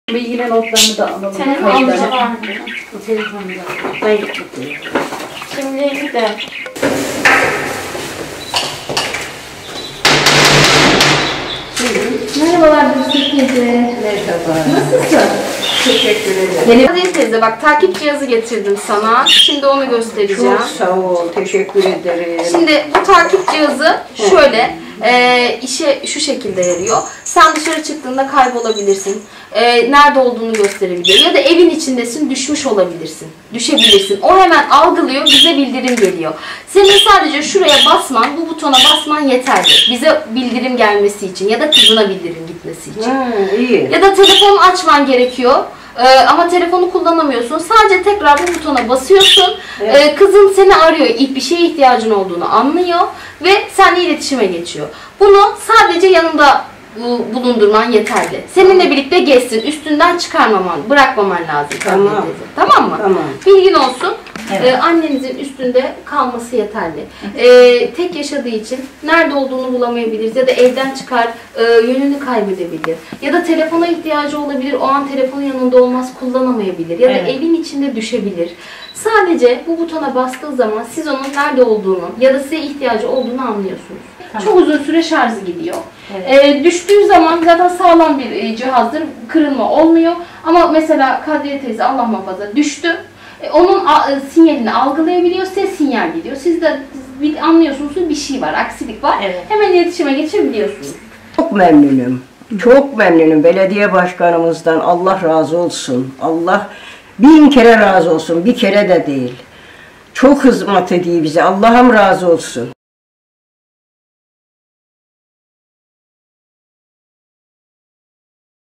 Sosyal Alarm Projesi
Pilot uygulama olarak ilk etapta 50 kişinin hizmetine sunulan proje kapsamında belirlenen vatandaşlara Trio Mobil Sosyal Alarm Cihazı verildi.